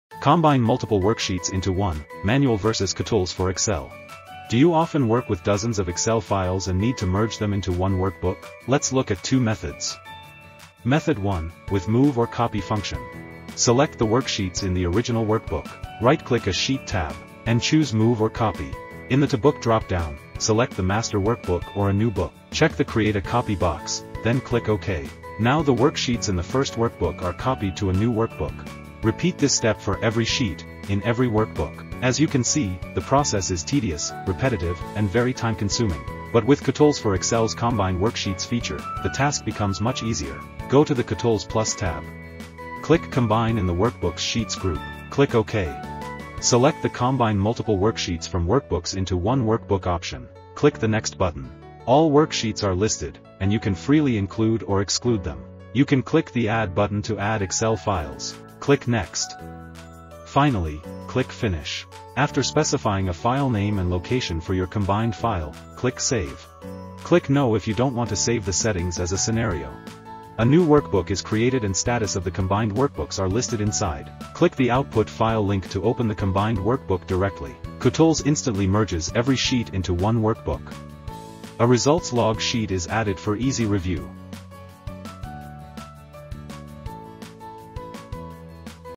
Excel Trick: Merge Worksheets from Workbooks in Seconds
Tired of manually merging Excel files one by one?
In this video, we compare two methods to combine multiple worksheets from different workbooks into one workbook:

Method A (Manual): Using Excel’s built-in Move or Copy function — slow and repetitive.

Method B (Kutools): Using the Combine Worksheets feature — fast, simple, and efficient.

See how Kutools for Excel can save you hours of work and prevent mistakes when dealing with large numbers of files.